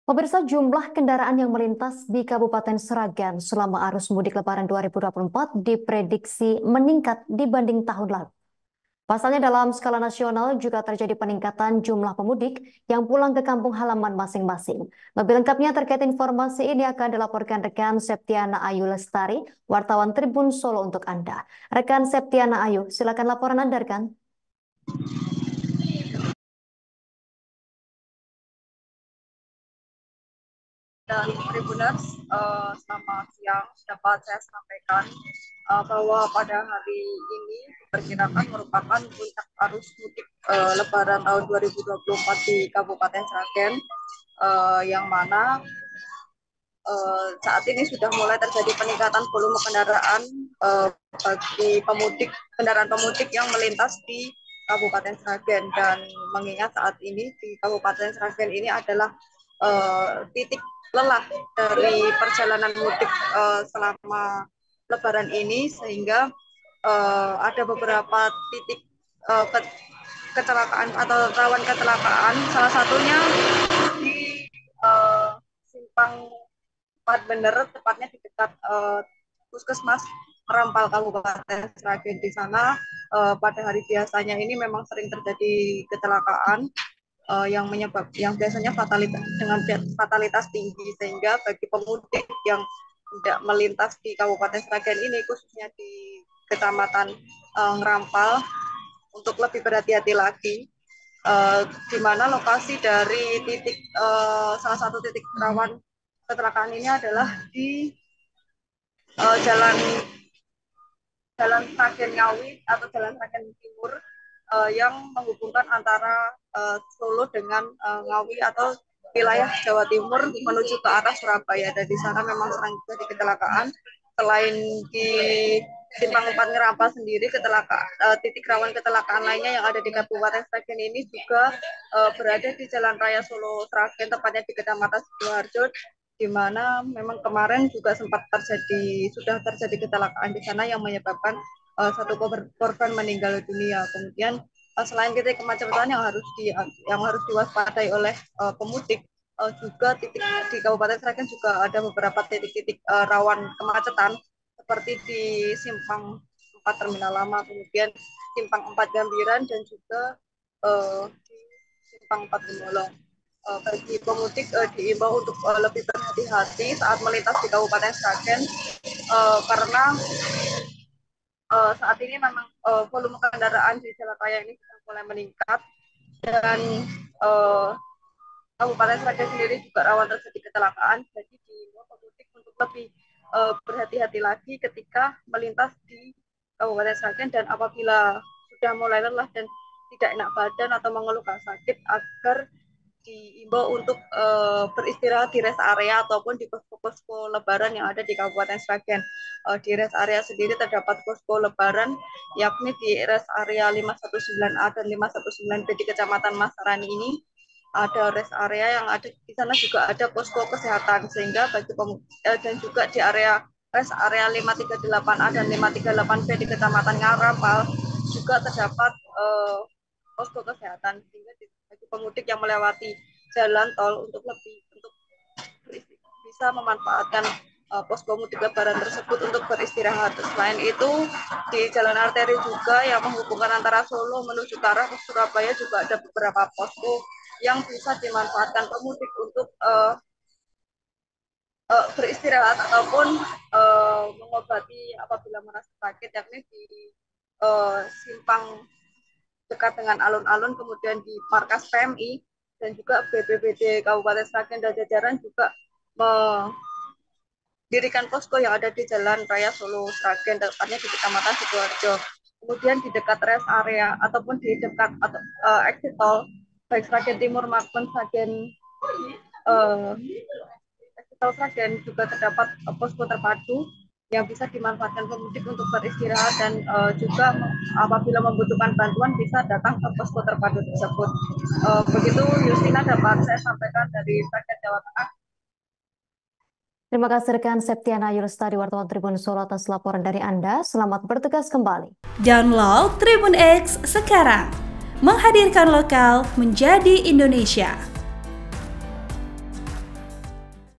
"Blackspot" Puskemas Ngrampal, Jalur Rawan Kecelakaan di Sragen, Pemudik Diimbau Hati-hati
Laporan wartawan TribunSolo.com-Septiana Ayu Lestari
TRIBUN-VIDEO.COM-Jumlah kendaraan yang melintas di Kabupaten Sragen selama arus mudik lebaran 2024 diprediksi meningkat dibanding tahun lalu.

Pasalnya, dalam skala nasional, juga terjadi peningkatan jumlah pemudik yang pulang ke kampung halaman masing-masing.(*)